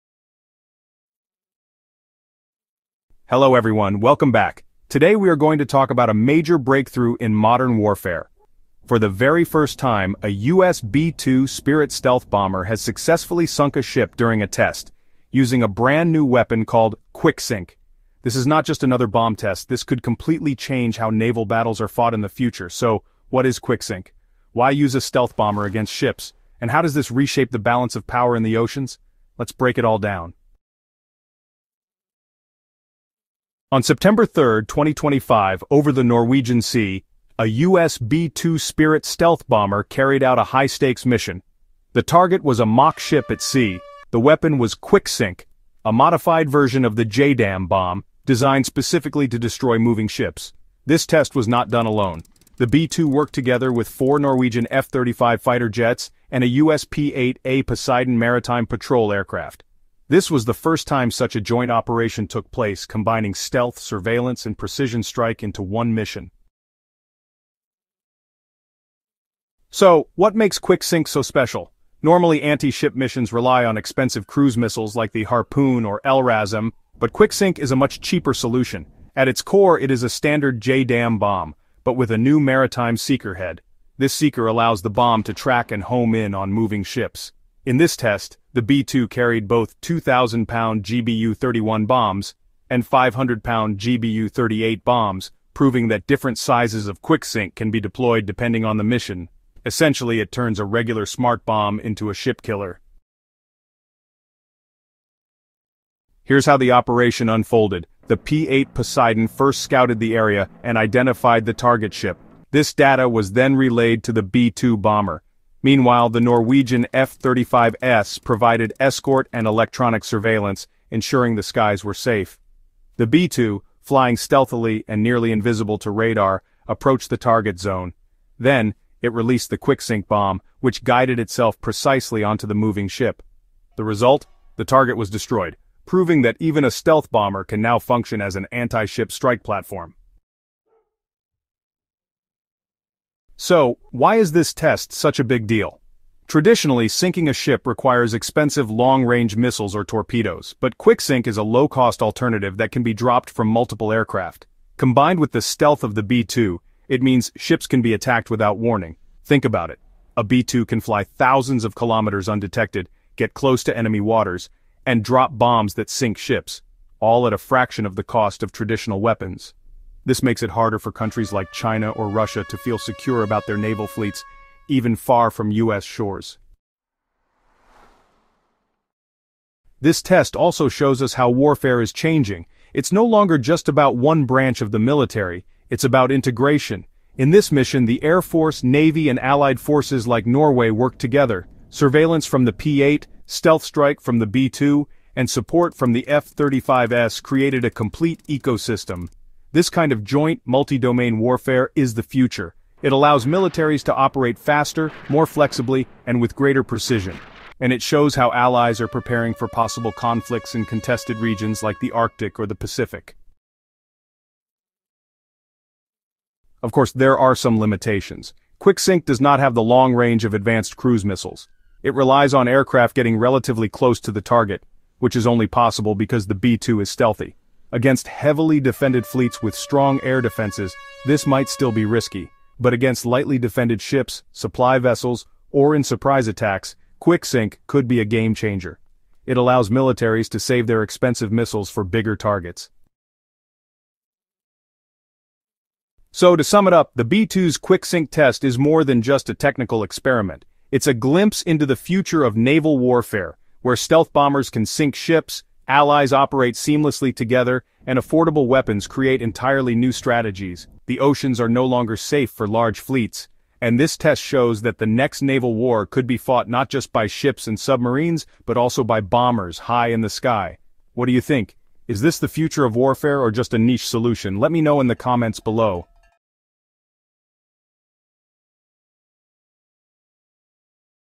B-2 Stealth Bomber Sinks Target in Anti-Ship Weapon Test
Prepare to dive into a groundbreaking moment in military technology with our latest video, "B-2 Spirit's Game-Changing QUICKSINK Test!" Discover how the U.S. B-2 Spirit stealth bomber successfully sunk a mock ship using the innovative QUICKSINK weapon during a high-stakes test over the Norwegian Sea. This revolutionary approach to naval warfare could redefine how battles are fought at sea.

Join us as we explore the details of this test, the capabilities of QUICKSINK, and the implications for future military operations. From joint missions to stealth tactics, learn why this could change the balance of power on the oceans.

OUTLINE:

00:00:00 A New Era in Naval Warfare
00:00:30 B-2 Spirit's Historic Mission
00:01:06 QUICKSINK Explained
00:01:42 How the Strike Worked
00:02:20 Strategic Importance
00:02:56 The Future of Warfare
00:03:33 Limitations and Conclusion
00:04:02 Final Thoughts